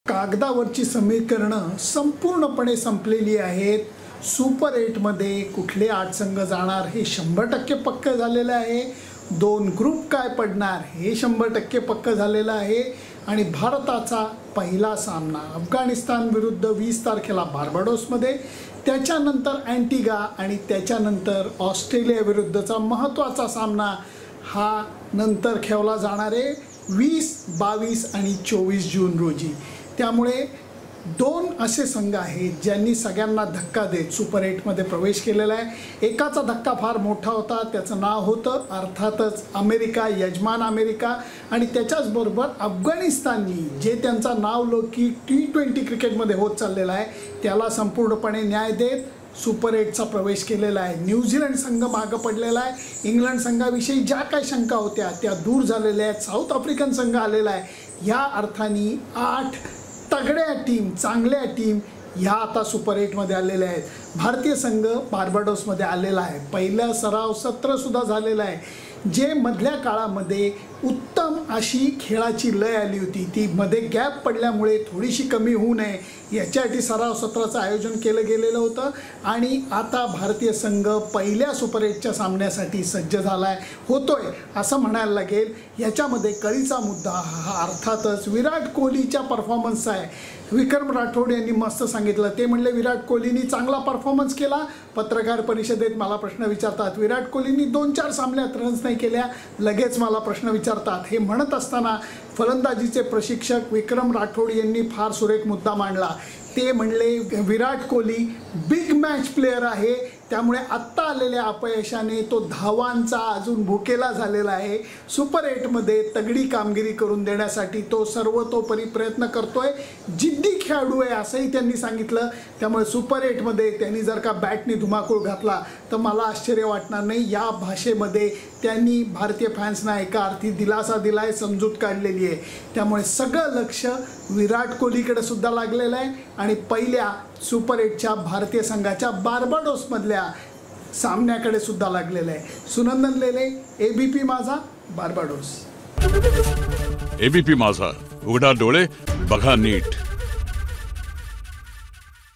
T20 World Cup : टीम इंडिया सुपर 8 च्या पहिल्या सामन्यासाठी सज्ज! विराट कोहली धुमाकूळ घालणार का?
Team India T20 : टीम इंडिया सुपर 8 च्या पहिल्या सामन्यासाठी सज्ज! विराट कोहली धुमाकूळ घालणार का?
Video Credit | Salman Shaikh/Producer | Krishnadas Bansode/ Editor